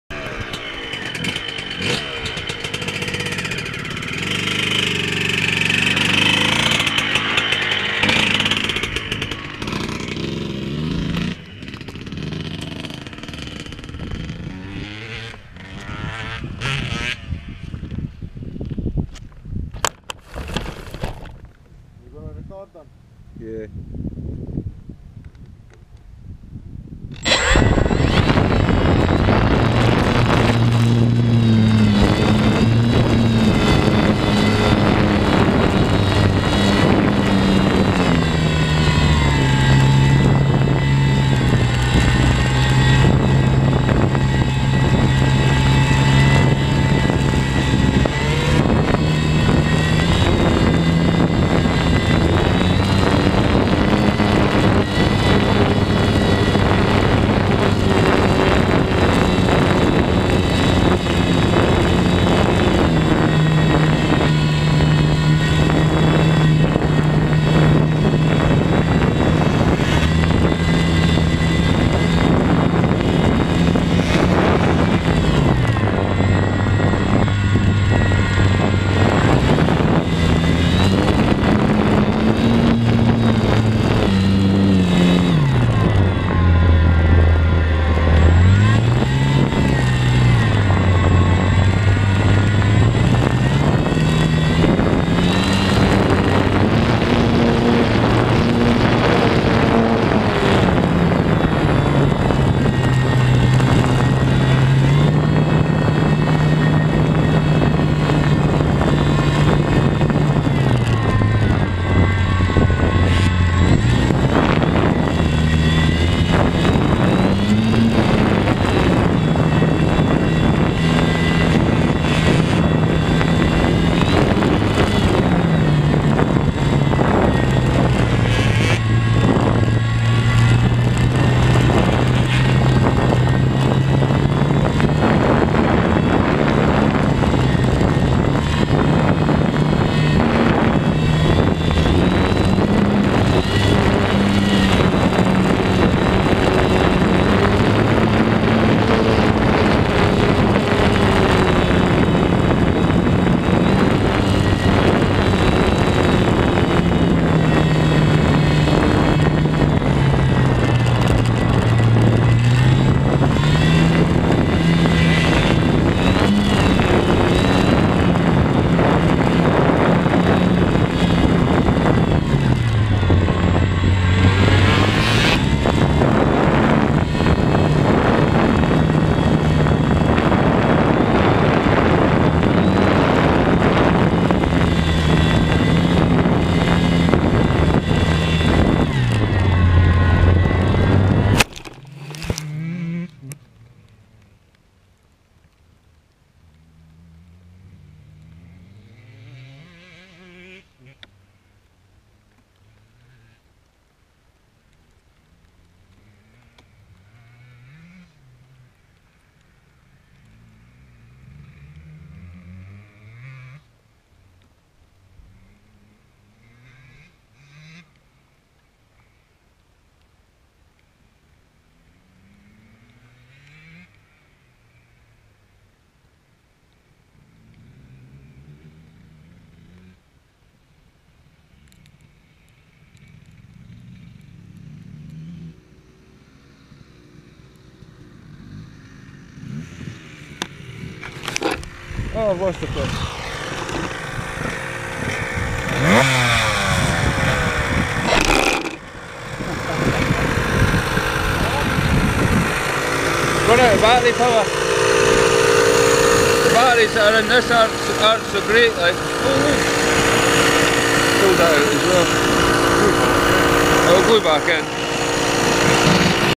flying up the farm on a camping weekend 464
fitted my gopro camera to the foam wot and started following the guys on there bikes